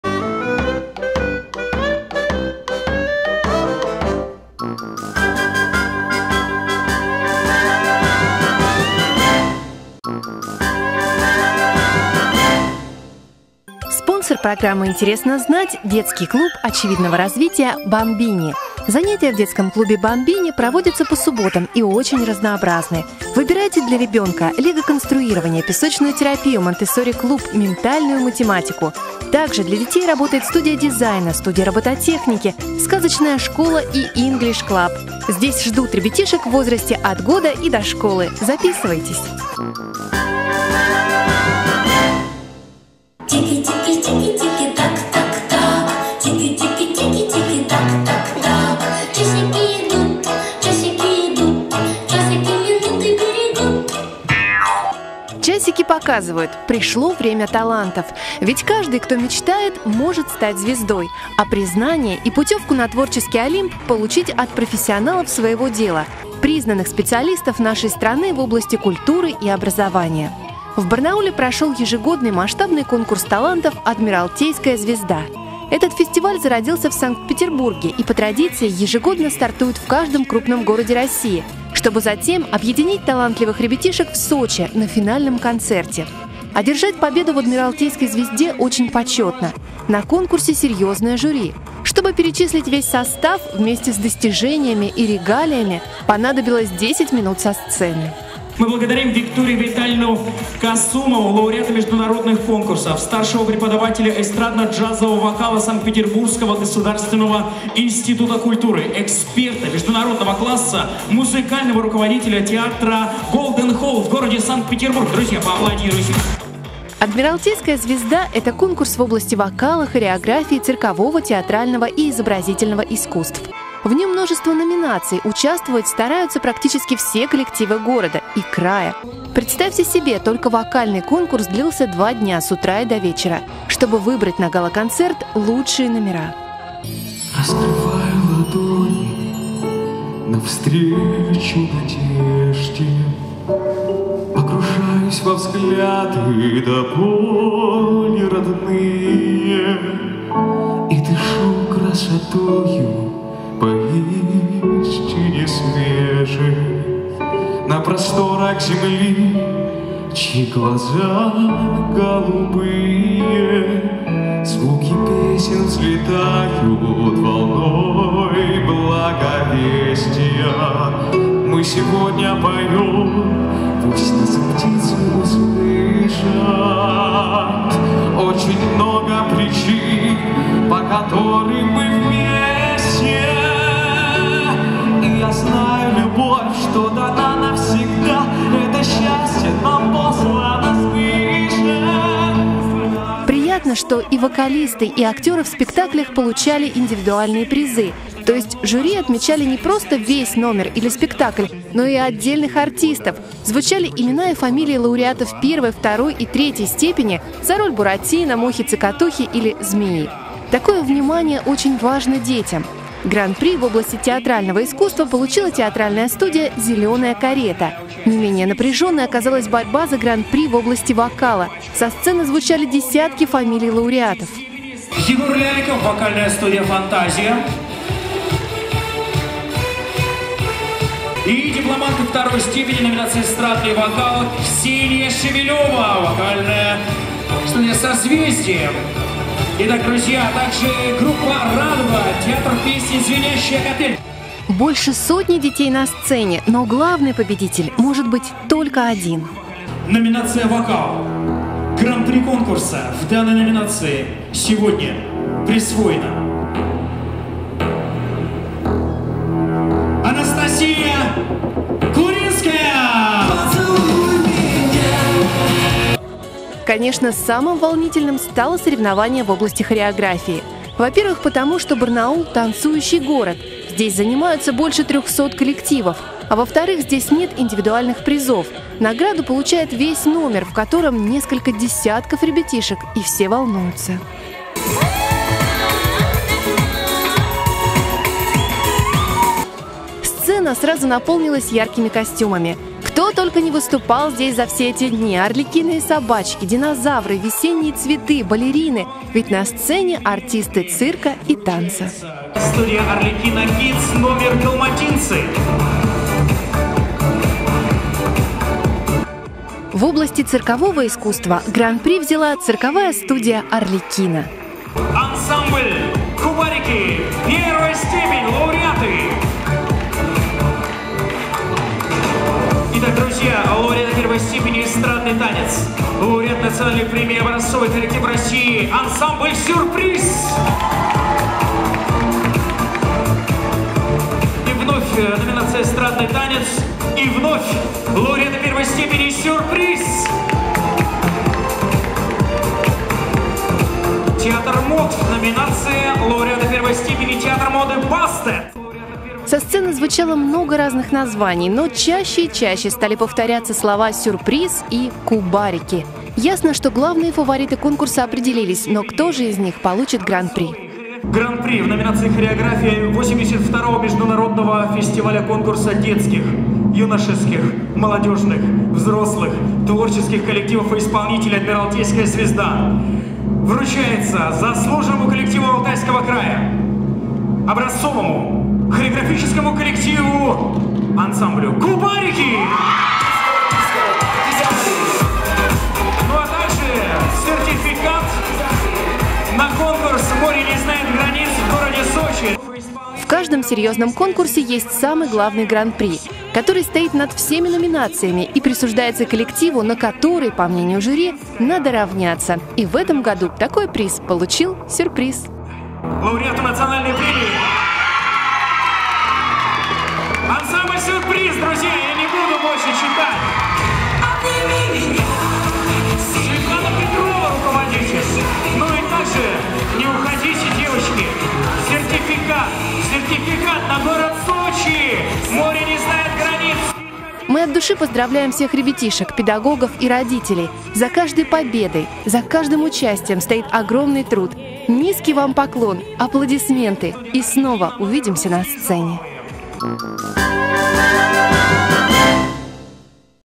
«Интересно знать»: конкурс «Адмиралтейская звезда»
Кто участвовал в конкурсе «Адмиралтейская звезда», ищите себя в выпуске. А для тех, кто не знает, что это, обязательно посмотрите! «Адмиралтейская звезда» — это масштабный конкурс в области вокала, хореографии, циркового, театрального и изобразительного искусства. Этот фестиваль зародился в Санкт-Петербурге и по традиции ежегодно стартует в каждом крупном городе России, чтобы затем объединить талантливых ребятишек в Сочи на финальном концерте. Одержать победу в «Адмиралтейской звезде» очень почетно. На конкурсе серьезное жюри. Чтобы перечислить весь состав вместе с достижениями и регалиями, понадобилось 10 минут со сцены. В конкурсе множество номинаций, участвовать стараются практически все коллективы города Барнаула (и не только). Представьте себе, только вокальный конкурс длился два дня с утра и до вечера, чтобы выбрать на гала-концерт лучшие номера.